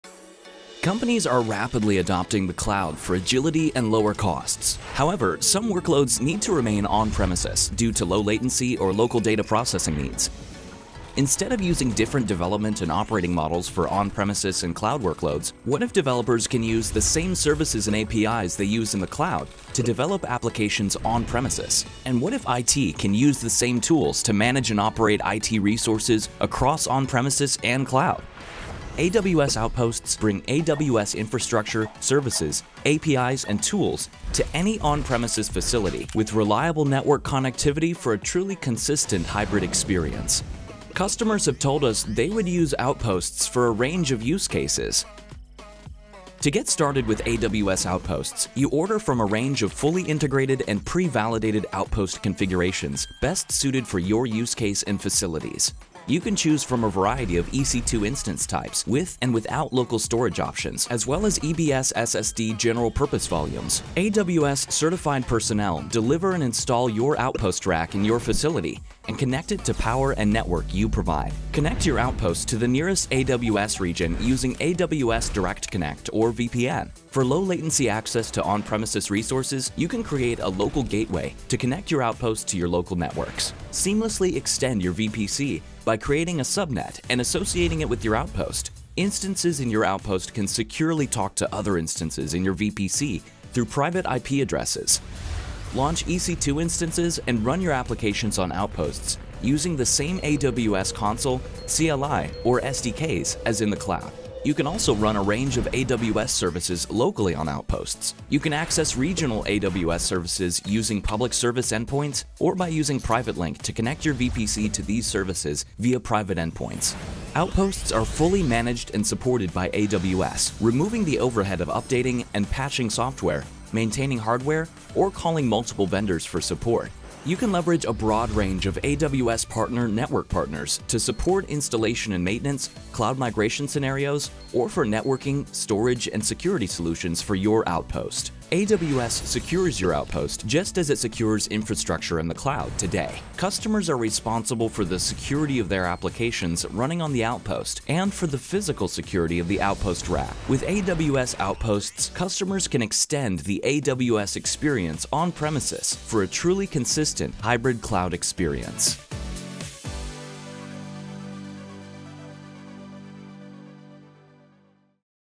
AWS Outposts: Overview and How It Works
AWS Outposts delivers AWS infrastructure and services to virtually any customer on premises facilities for a truly consistent hybrid experience.